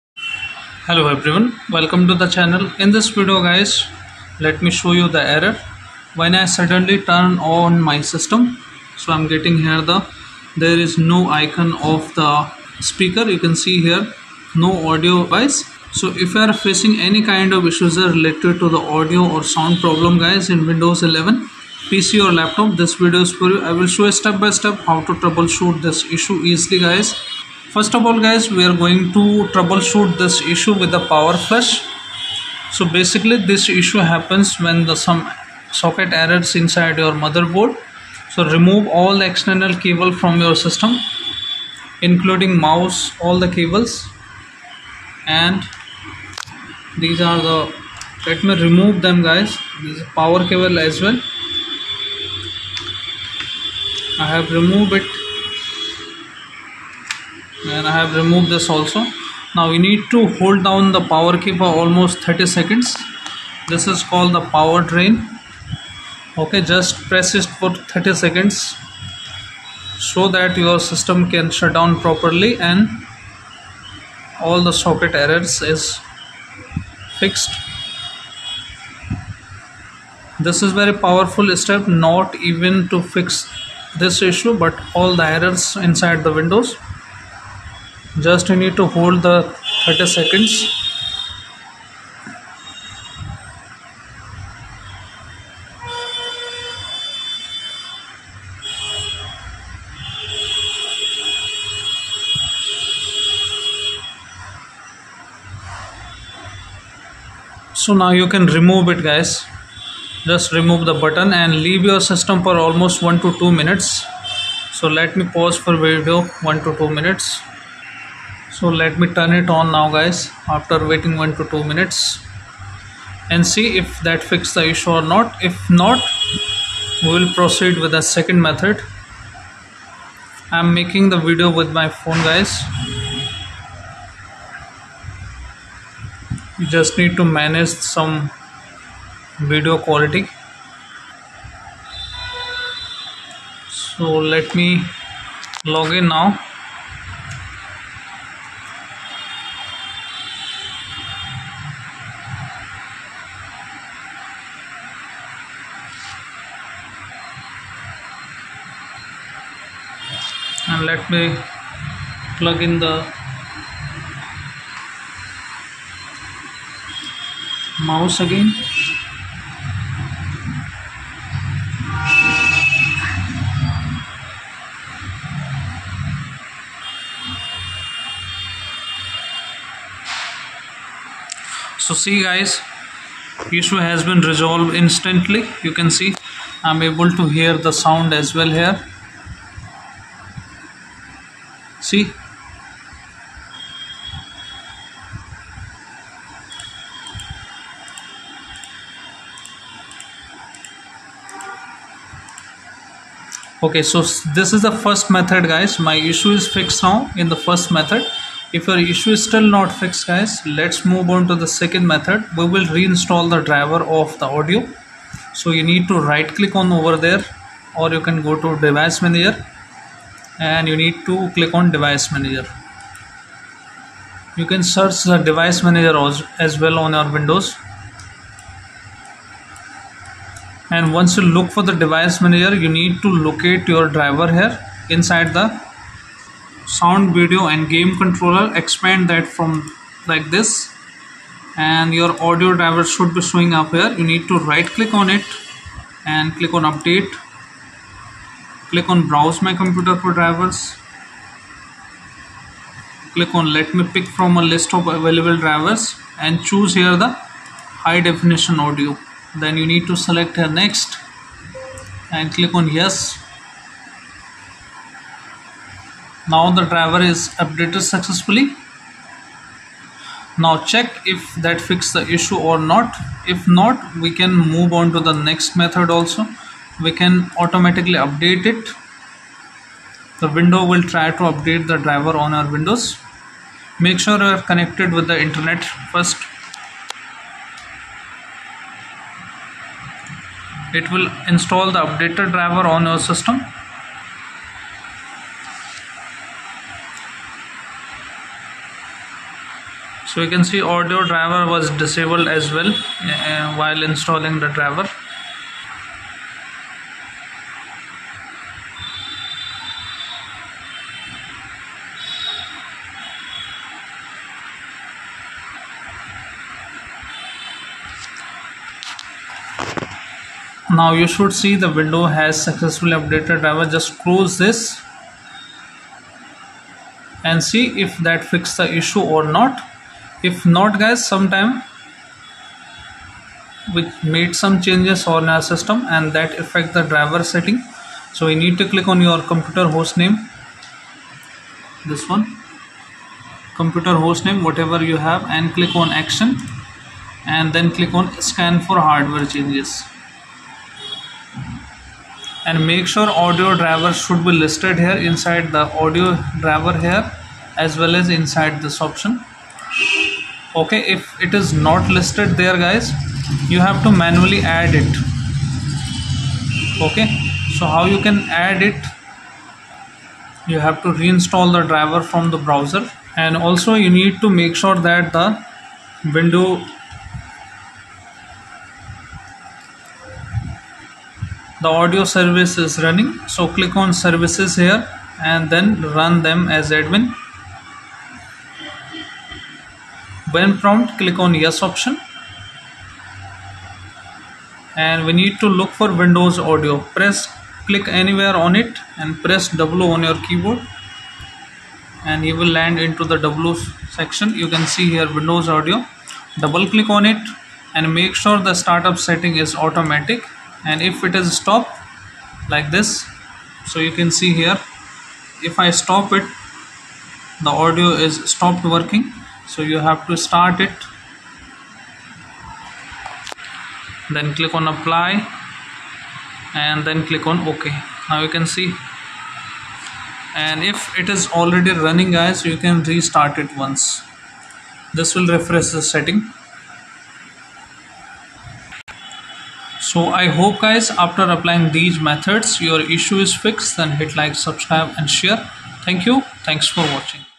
(✅4 Fixes -2024) "Audio or Sound Problems" In windows 11 Laptop & PC | No Audio Device is Installed?
✅ This instructions in this video tutorial can be applied  for  laptops, desktops, computers, and tablets which run the Windows 11 operating systems. This tutorial will work on all hardware manufactures, like Dell, HP, Acer, Asus, Toshiba, Lenovo, and Samsung.